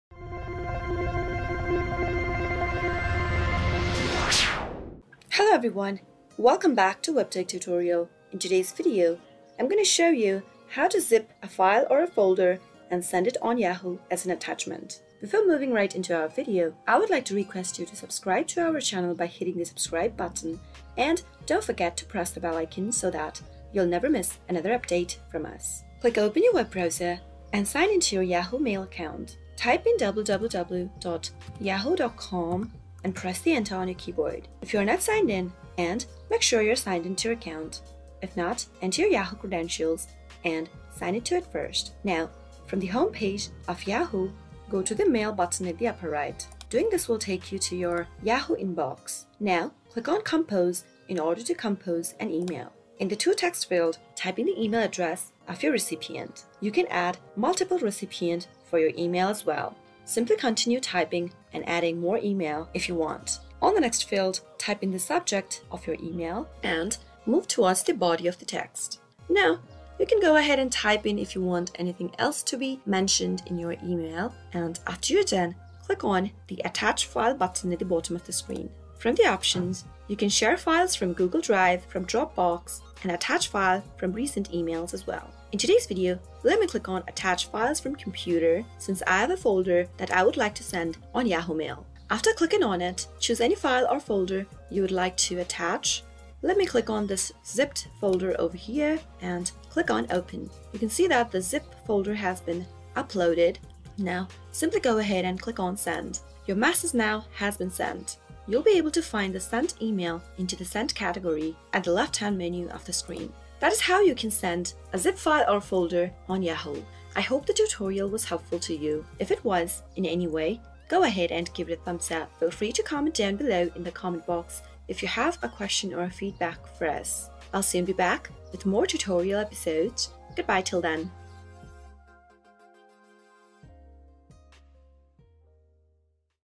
How to Send a Zip File Folder on Yahoo Mail?
Don’t know how to zip a file folder and send it on Yahoo? Here we bring you step by step process to make it simple for you to learn how you can easily zip file folder and send on yahoo.

Yahoo Mail limits the size of emails you can send to 25 MB. Provided your folder and the contents of your email are no larger than that, you can send a zipped folder as a Yahoo Mail attachment.
In order to send a zip file follow the instructions below:
1: Go to your web browser and type in ‘yahoo.com’ and sign in to your account
2: Click on the ‘mail’ button on the upper right
3: Click on ‘compose’
4: Enter the emails that you want to send the zip file to, add as many recipients as you want
5: Type in your subject and compose your email
6: After you are done Click on the ‘attach’ icon
7: Click on ‘attach files from computer’ or any other option you like
8: Select your zip file or folder and click on ‘open’
9: Finally Click on ‘send’ and your zip file folder will be simply sent on yahoo.